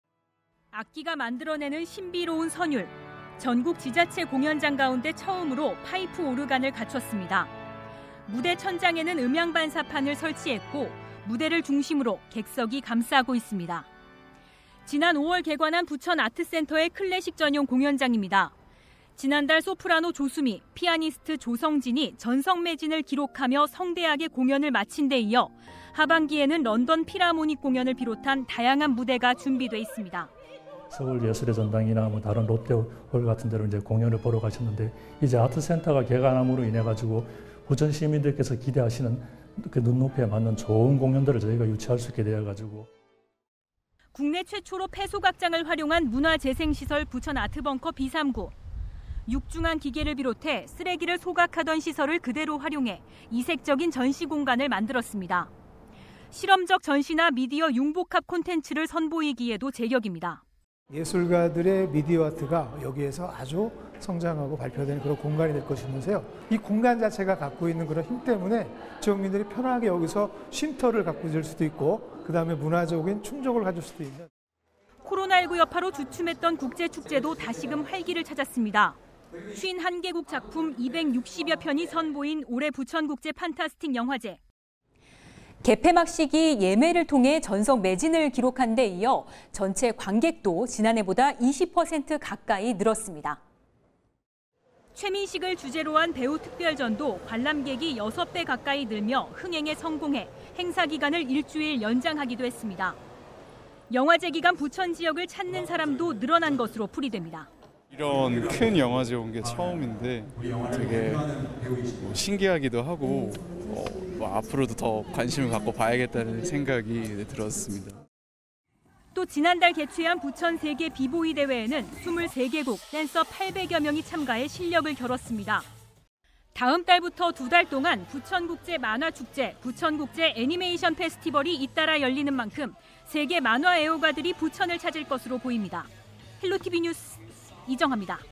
클래식부터 국제 축제까지…문화가 밥 먹여주는 부천
경기 부천은 오랜 기간 문화도시를 표방하고 있습니다.
클래식 공연과 전시부터 영화·만화 등 콘텐츠를 중심으로 한 국제 축제까지 연간 문화 행사가 그야말로 한손에 꼽기 어려울 정도인데요.
이 때문에 지역을 찾는 사람들도 늘어나면서 부천의 문화 산업이 꾸준한 성장세를 이어가고 있습니다.